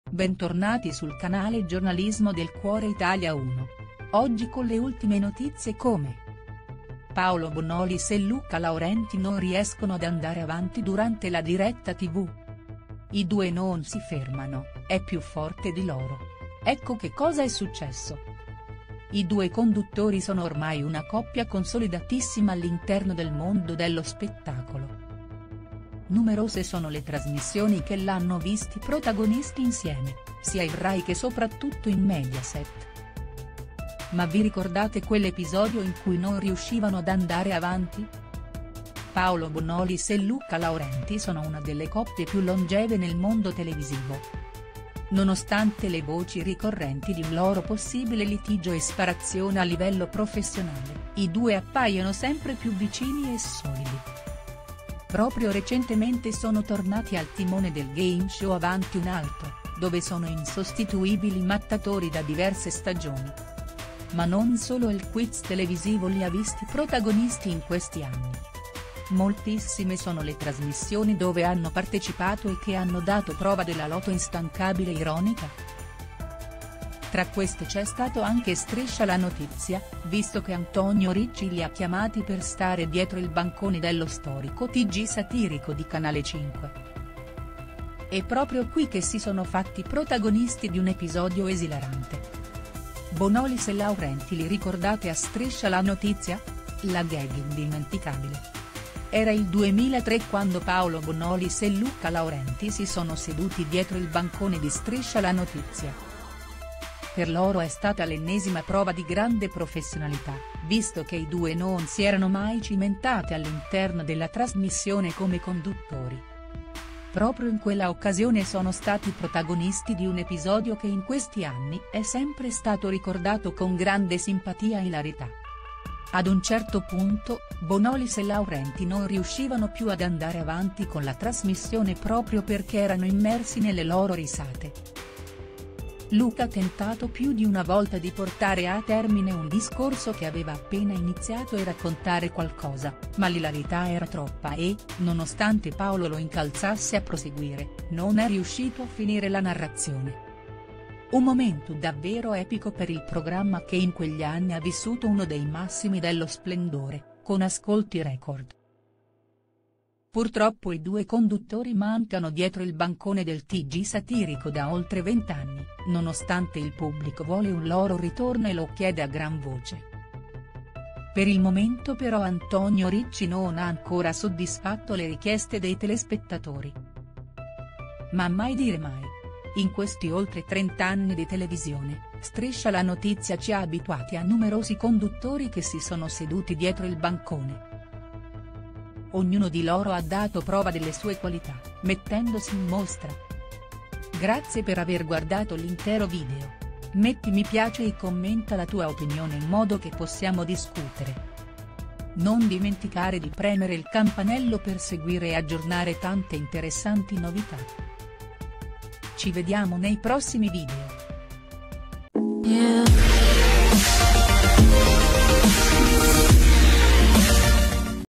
Bonolis e Laurenti, panico in diretta non riescono a continuare
Paolo Bonolis e Luca Laurenti non riescono ad andare avanti durante la diretta tv. I due non si fermano, è più forte di loro. Ecco che cosa è successo.

I due conduttori sono ormai una coppia consolidatissima all’interno del mondo dello spettacolo. Numerose sono le trasmissioni che l’hanno visti protagonisti insieme, sia in Rai che soprattutto in Mediaset. Ma vi ricordate quell’episodio in cui non riuscivano ad andare avanti?